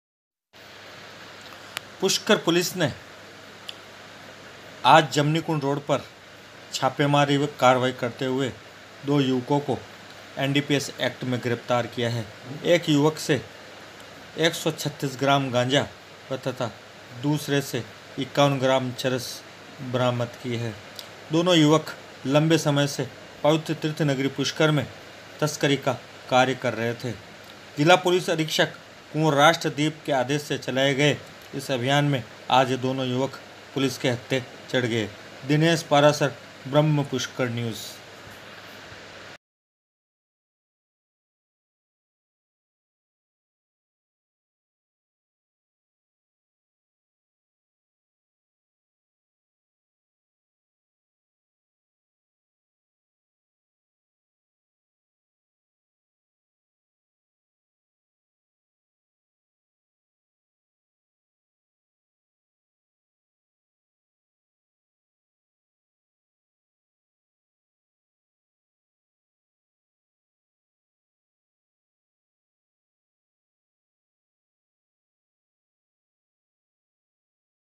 चरस गांजे सहित दो युवक गिरफ्तारएक हैआलनियावास का तो दूसरा है सथाना गांव नागौर का रहने वाला
पुष्कर 27 जुलाई --पुष्कर पुलिस को फिर मिली बड़ी सफलता*

*मादक पदार्थों की तस्करी करते हुए दो युवकों को किया गिरफ्तार*

  पुष्कर  पुलिस को आज एक और बड़ी सफलता मिली मादक पदार्थों की धरपकड़ हेतु चलाए जा रहे अभियान के तहत  आज फिर   पुलिस ने दो युवकों को मादक पदार्थ सहित गिरफ्तार किया है दोनों युवकों से 136 ग्राम गांजा व 51 ग्राम चरस बरामद की सीआई हेमेंद्र शर्मा ने बताया कि एसपी कुंवर राष्ट्रदीप के निर्देशन एवं अतिरिक्त पुलिस अधीक्षक ग्रामीण सुनील कुमार वह सीओ ग्रामीण विनोद कुमार सीपा के सुपर विजन में चलाये जा रहे अभियान के तहत  दआज दो युवको को जमनीकुण्ड रोड नाला फीडर  से गिरफ्तार कर अवैध चरस व गांजा बरामद किया है थाना अधिकारी ने बताया कि आज मुखबिर की सूचना के आधार पर जमनी कुंड फीडर नाला से दीपक शर्मा पुत्र अमरचंद जाति ब्राह्मण उम्र 31 साल निवासी चारभुजा नाथ मंदिर के पास आलनियावास थाना थांवला जिला नागौर हाल शिव जी गुर्जर का मकान जमुनी कुंड रोड सामरिया कॉलोनी पुष्कर को 135 ग्राम अवैध मादक पदार्थ गांजा  के साथ व गोपी सिंह पुत्र नोरत सिंह जाति रावणा राजपूत उम्र 35 साल निवासी ग्राम साथाना, थाना पादु कला जिला नागौर हाल  स्टार होटल जमुनीकुंड रोड पुष्कर जिला अजमेर को अवैध मादक पदार्थ 51 ग्राम चरस के साथ में मोटरसाइकिल  सहित गिरफ्तार किया गया है जिस के संबंध में मांगलियावास थानाधिकारी सीताराम को जांच सौंपी गई है टीम में सीआई  हेमेंद्र शर्मा के अलावा सहायक निरीक्षक निरीक्षक नाथूलाल कांस्टेबल सुनील कुमार अमित शिवकरण सुखाराम थे। फोटो कैप्शन पुष्कर में पुलिस के हत्थे चढ़े दो तस्कर दीपक व गोपी सिंह फोटो दिनेश पाराशर पुष्कर। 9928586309